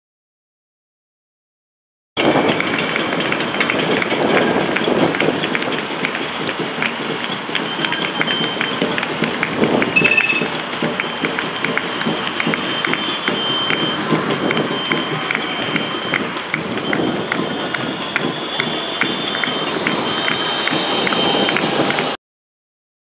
34. Maraton Warszawski - Video
My first start in marathon. It's about 39. km